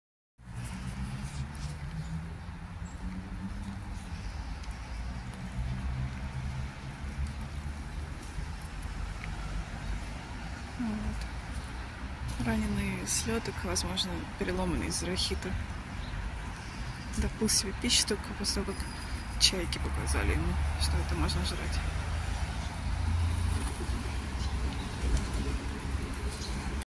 Толпа голубей и вороний слёток (2)
24.09.23 помойная еда (открытые баки) и остальные антропогенные факторы, способствующие синантропизации, приводят к процветания вида, но к ухудшению качества отдельных особей...